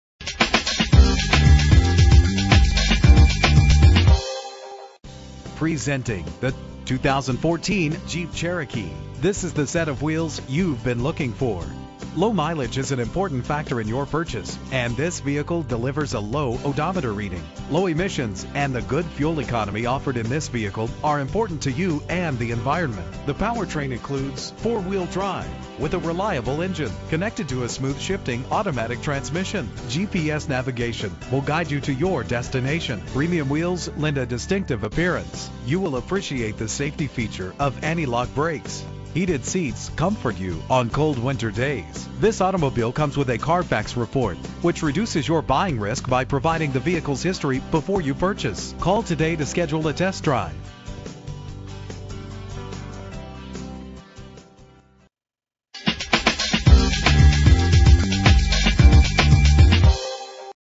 2014 Jeep Cherokee - Brighton MI
Year: 2014
Make: Jeep
Model: Cherokee
Trim: Limited Edition
Engine: 2.4 L, 4 cylinder
Transmission:
Color: White
Mileage: 11000
Address:
8240 West Grand River